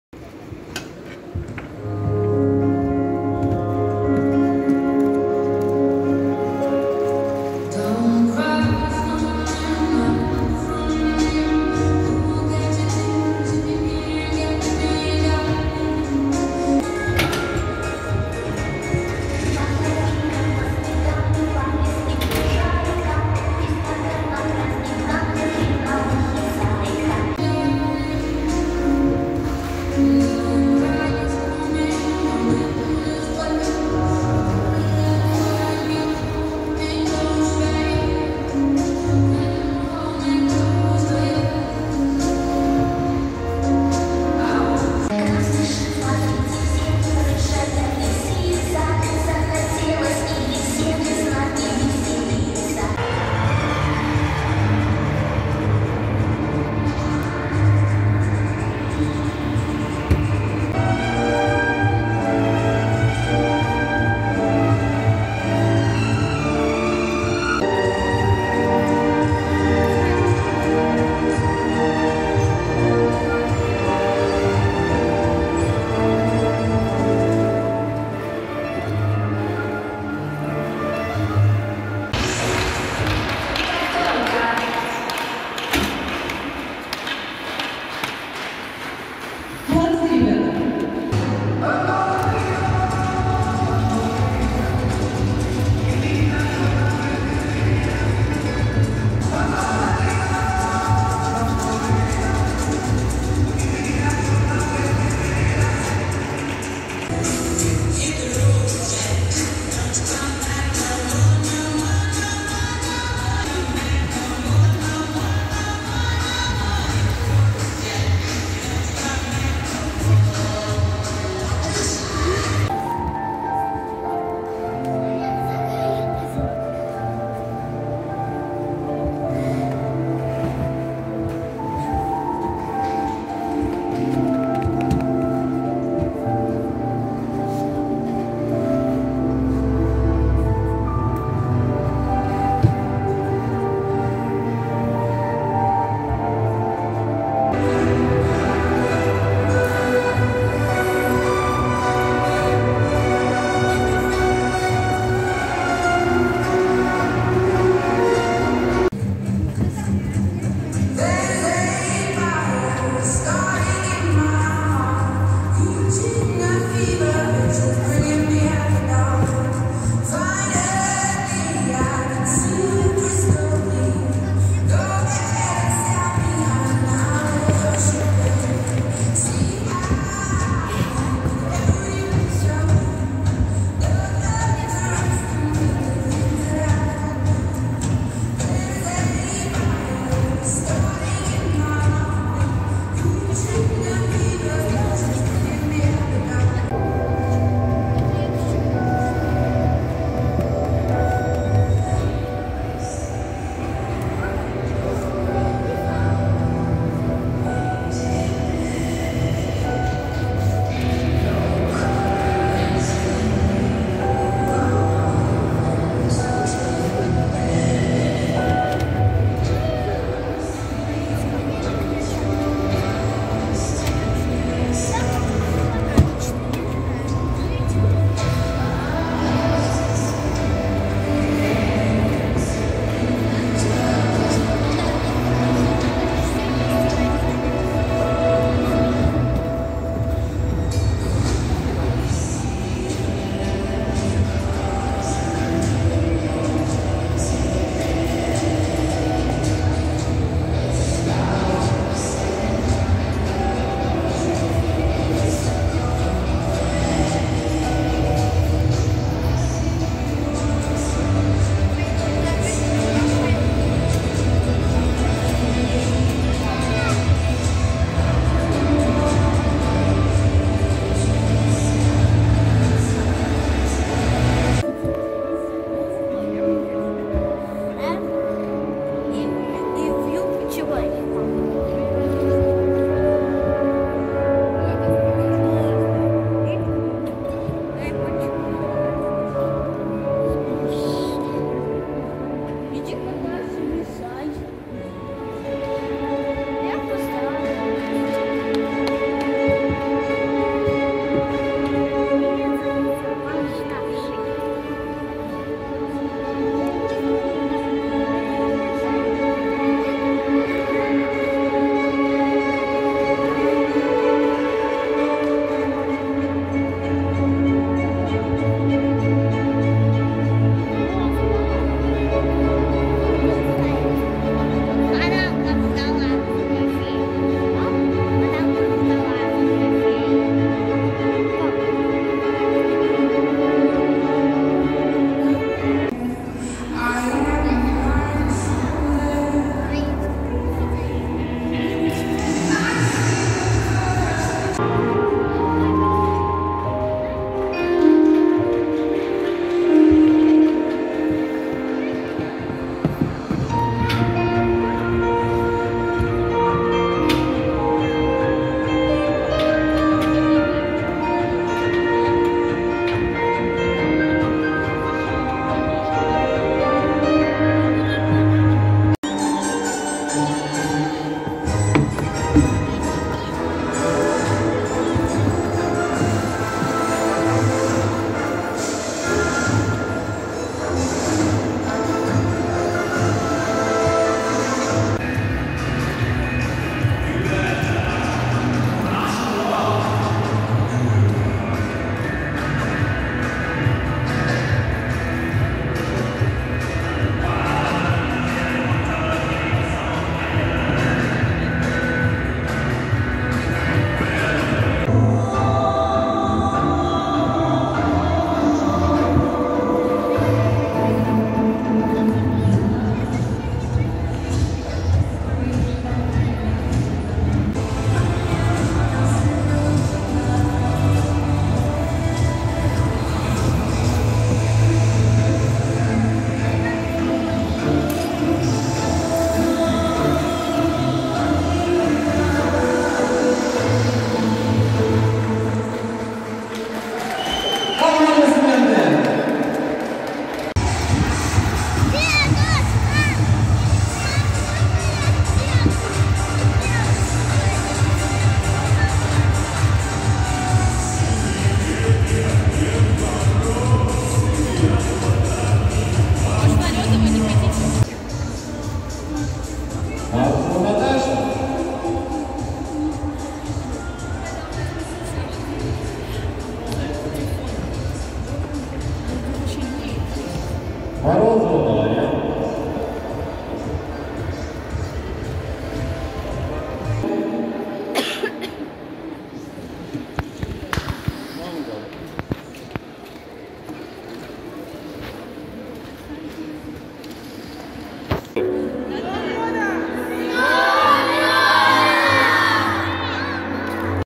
Фигурное катание ФОК 31.12.2022г
Новогоднее ледовое шоу г.Кулебаки
Видео: Алексея Комзикова